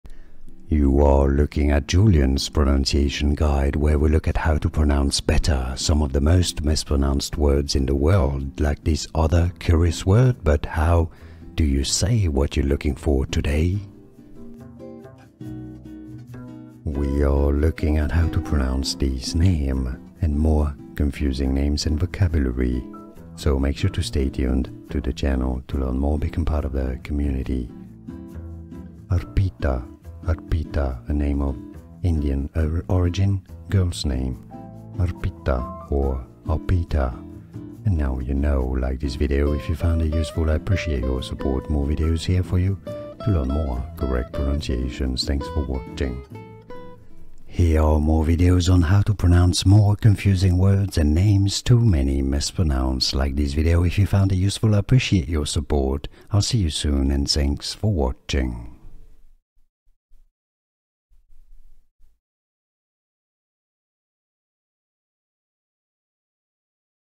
How to Pronounce Arpita (correctly!)
Welcome! Here you will hear How to Pronounce Arpita (correctly!) i.e. the “correct” pronunciation.

You can skip the intro through the time stamps below:
00:00 - Pronunciation Intro
00:15 - How to Pronounce Arpita (correctly!)